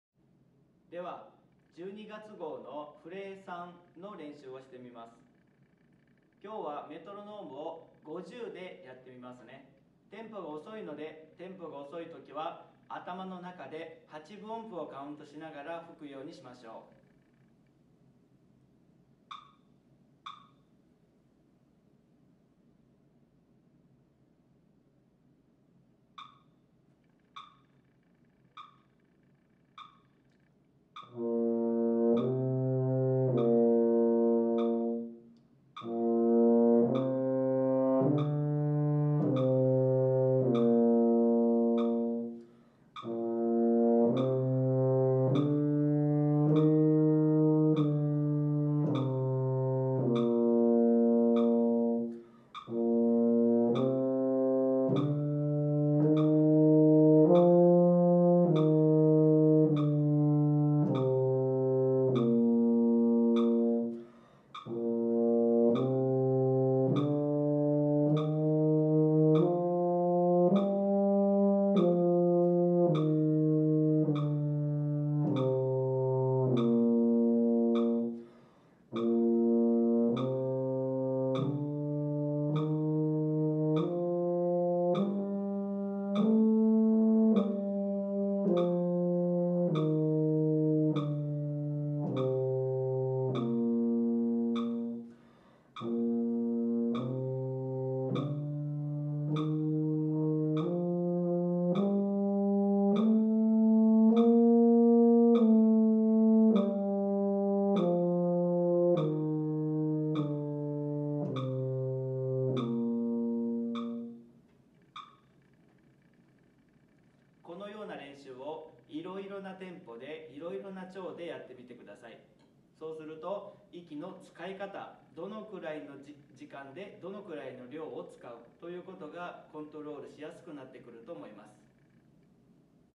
BJ-Euph-21
バンドジャーナル2014年1月号、ユーフォニアム・ワンポイントレッスンの補助動画です。
ぜひ参考にしてくださいね！

阿部 竜之介 オフィシャルウェブサイト